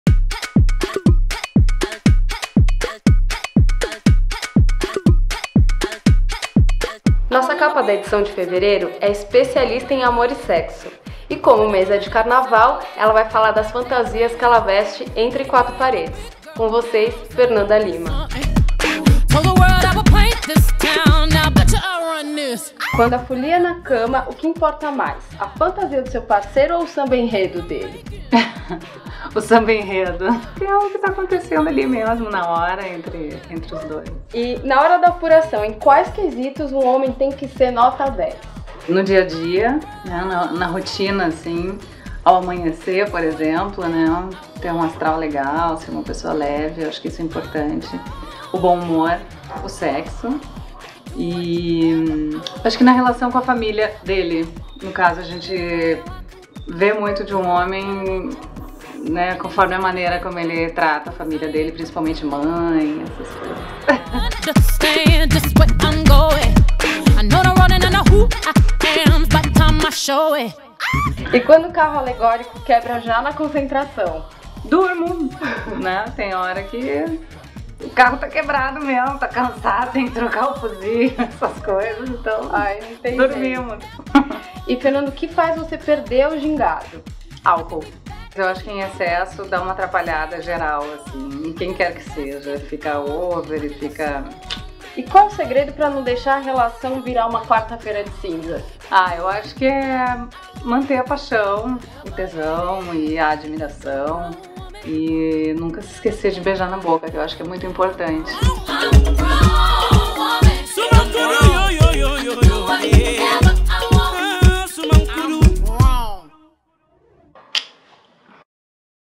Nos bastidores do shooting de fevereiro da Marie Claire, Fernanda Lima fala amor, sexo e Carnaval
Capa de Marie Claire de fevereiro, a apresentadora abriu o jogo sobre sua intimidade e contou o segredo para a relação não virar uma Quarta-feira de Cinzas, ou seja, não cair no tédio: “Nunca esquecer de beijar na boca.”